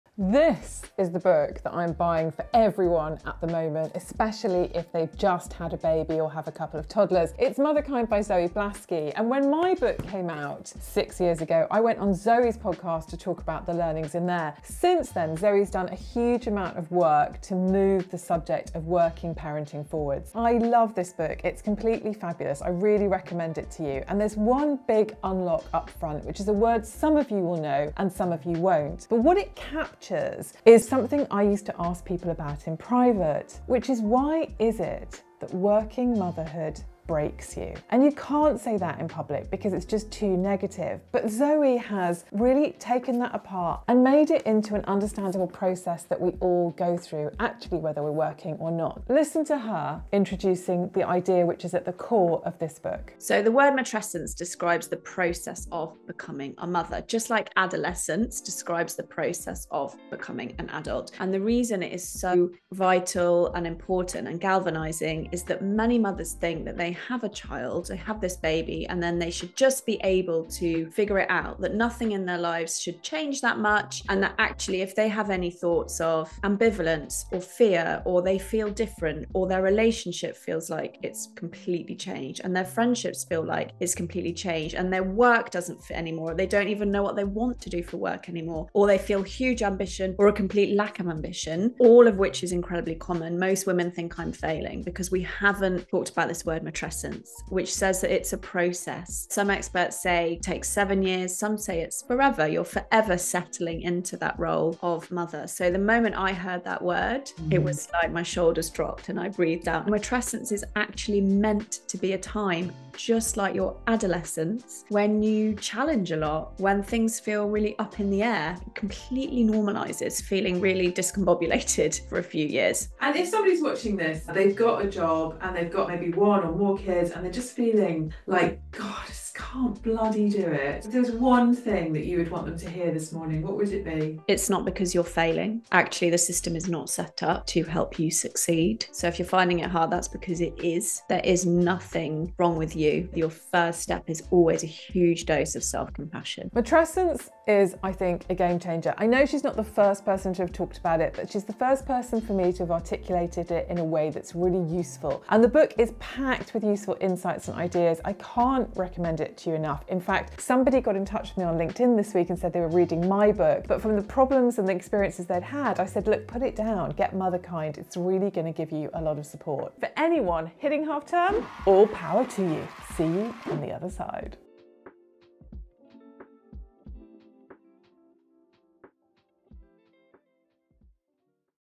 Does working motherhood have to break us? ❤️‍🩹
I used to say it did.

But a fab new book is out by Zoe Blaskey called Motherkind and her opener was a total re-set for me.